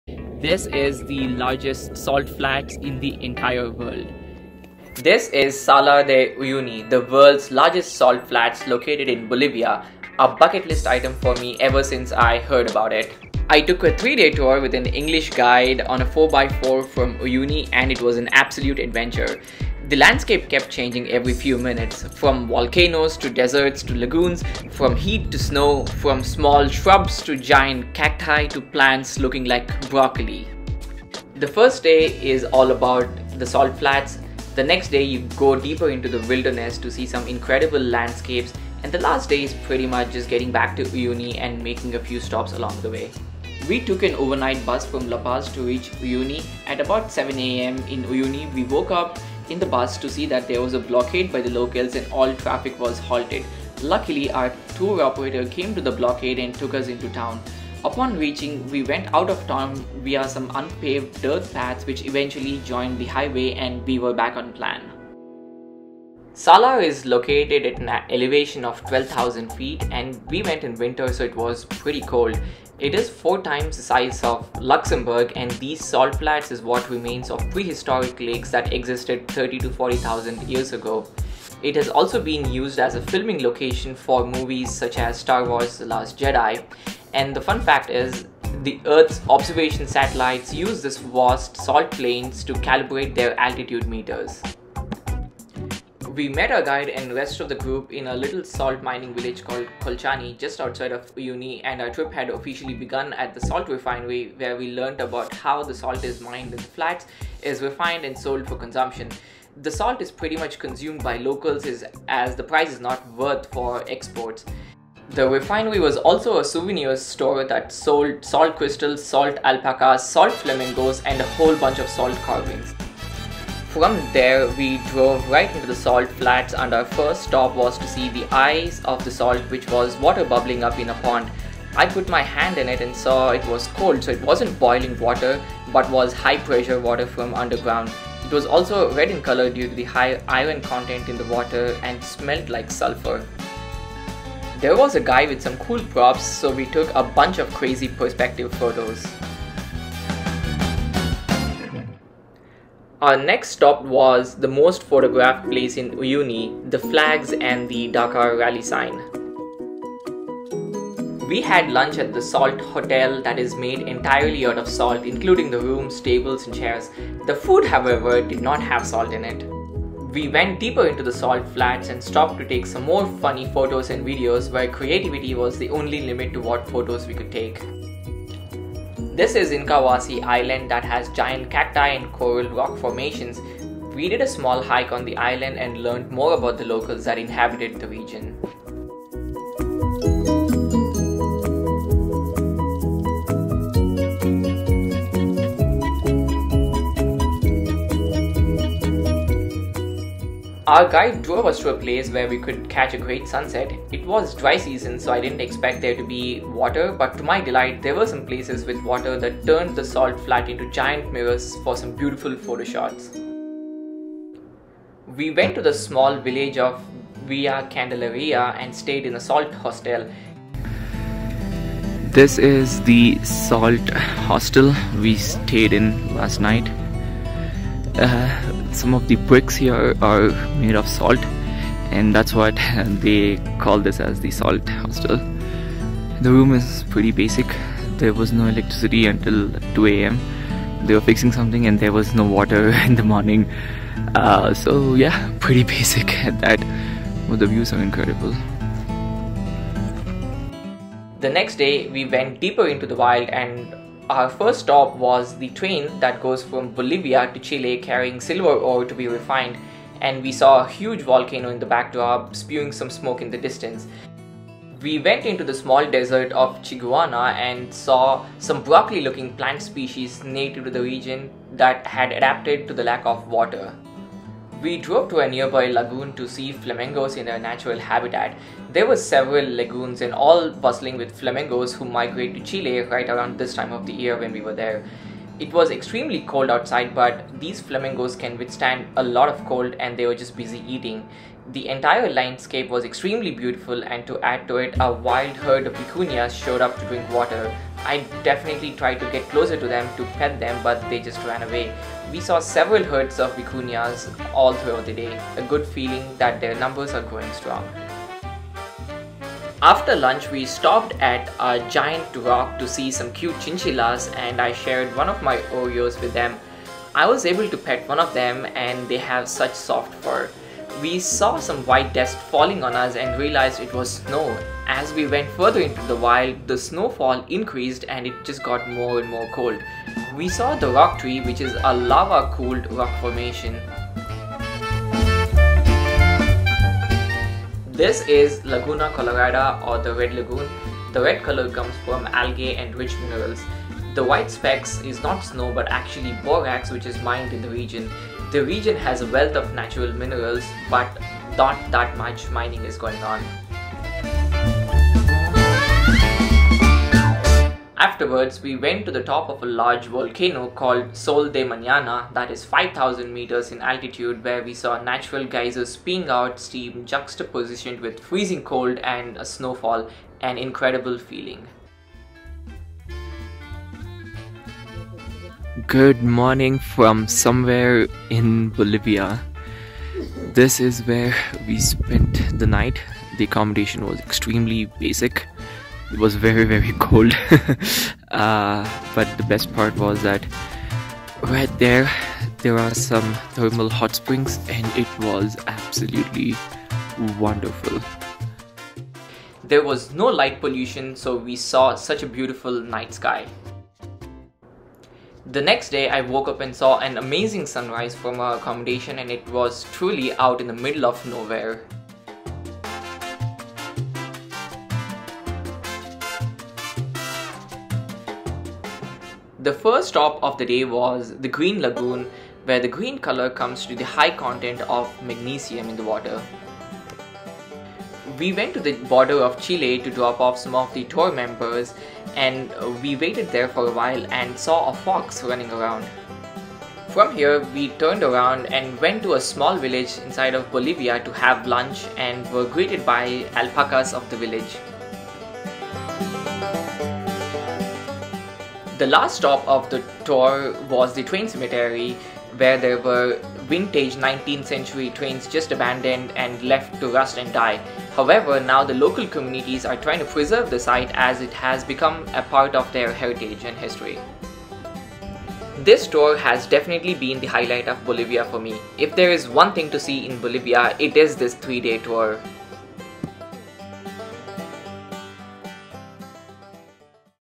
World's Largest Salt Flats | Highlight of Bolivia | Salar De Uyuni
3 days trip to the world's largest salt flats, Salar de Uyuni which is located in Bolivia, South America.

Travel Itinerary

Day 1: Salt Flats, Fish Island, Salt Hostel, Colchani

Day 2: Ollagua volcano, Red lagoon, Rock Tree, Sol De Mañana volcano, Hot springs, Geysers

Day 3:  Chile Border, Green Lagoon, Train Graveyard